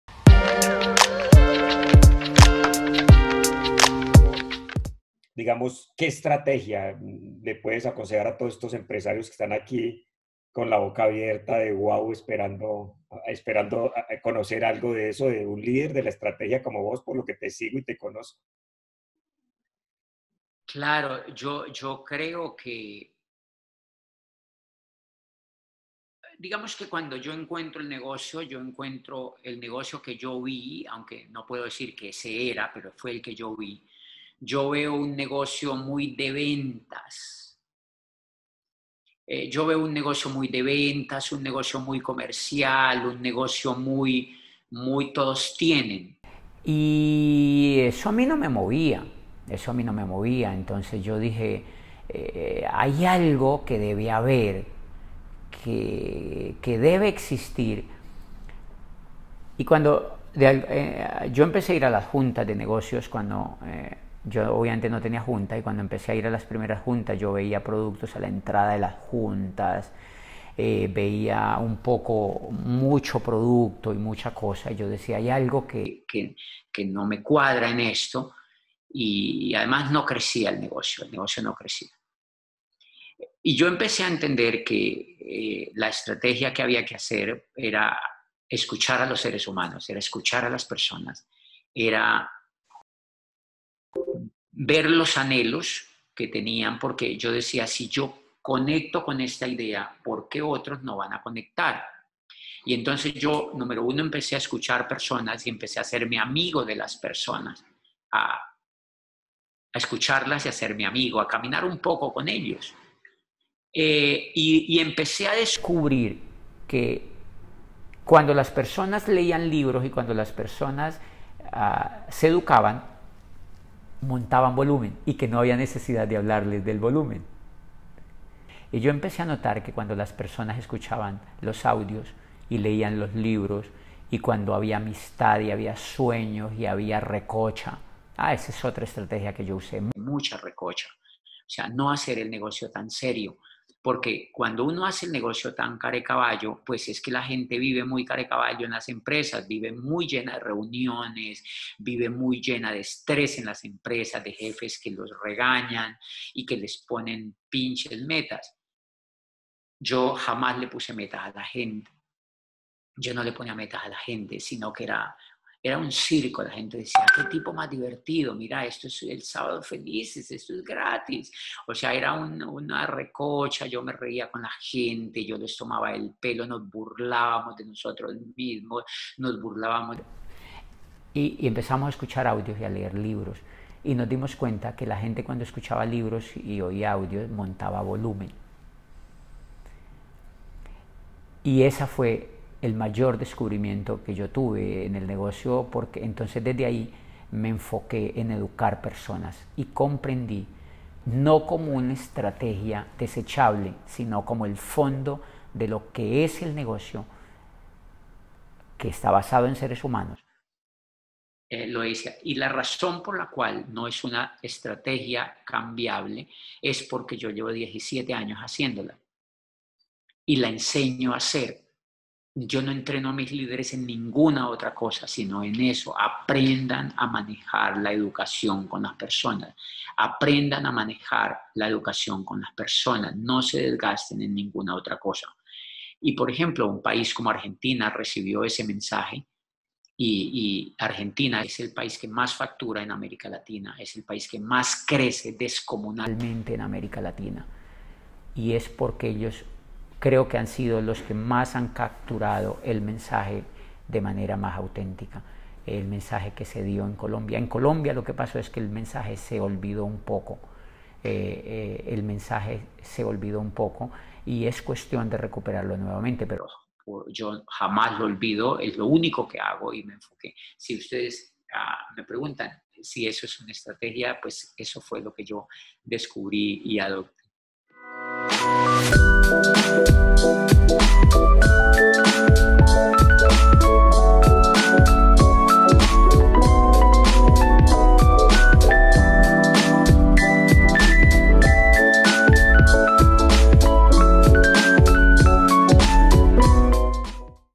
COMO CALIFICÓ JOSE BOBADILLA A EMBAJADOR CORONA?
Una de las preguntas que siempre me hacen es cuales son las estrategias que usé para calificar Embajador Corona o cuales fueron los trucos secretos para construir mi negocio de network marketing. Lo resumo en 5 minutos.